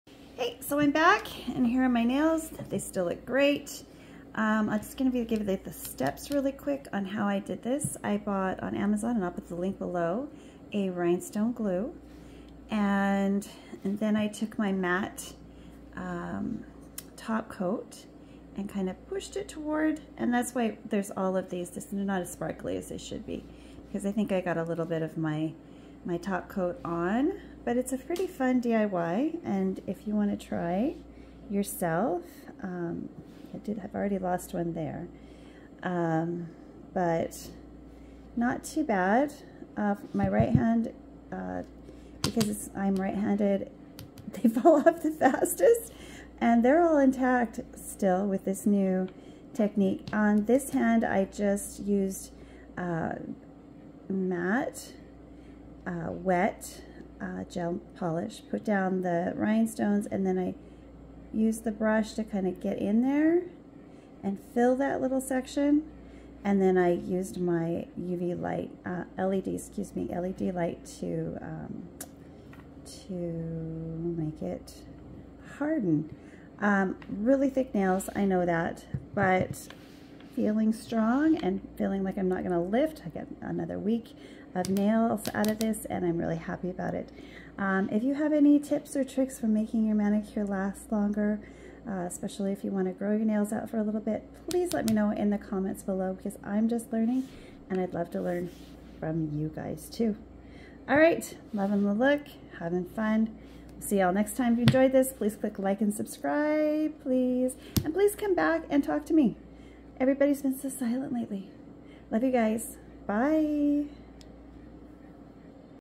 Directions for a backfill using embeds! Fun nail design extension.
I have been looking for a fun way to extend the life of my nail designs while at the same time protecting my natural nails from dreaded lift (bad). My rhinestones in this design originally lasted about two days.  While looking for ways to reattach the bling, I realized they would just catch on clothing and fall off.  A few weeks later, my natural growth gave me an 💡.  Not only did the diamonds stay, but there was the added benefit of a longer period of time with the color and design, which I love.

Rhinestone Glue...

Matte/Shiny Base Gel Polish...